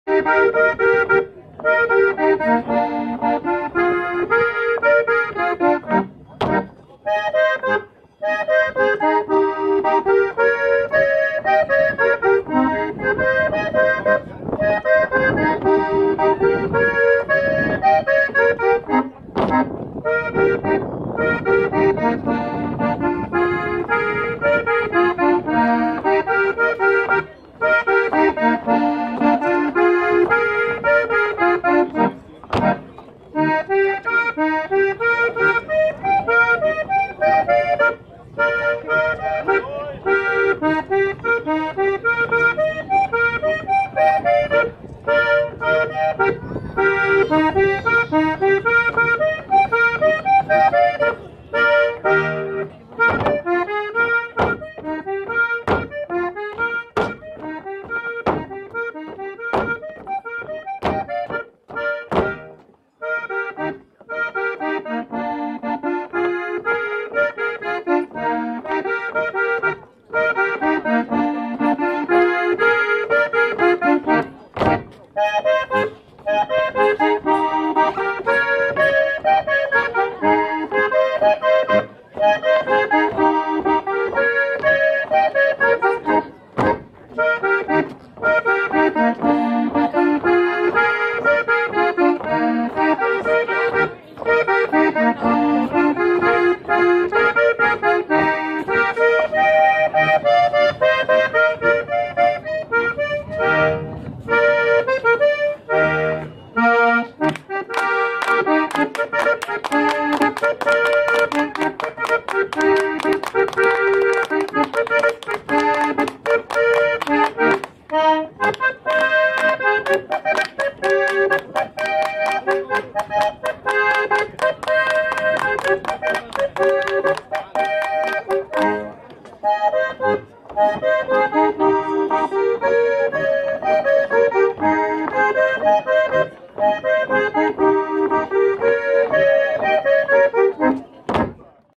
Austrian folk dance Landler (Ramsau) / Австрийский лендлер
Лендлер ( Landler ) – старинный крестьянский парный круговой танец. В разных провинциях и регионах танец немного отличается, везде  свои особенности.  Он исполняется в размере ¾ и для него характерен скользящий шаг, вращения партнерши, а также одновременное кружение обоих партнеров в разных направлениях. Долгое время лендлер подвергался гонению со стороны церкви, поскольку, близость танцующих в лендлере считалась безнравственной, а взять партнершу за руку считалось нарушением всех правил приличий.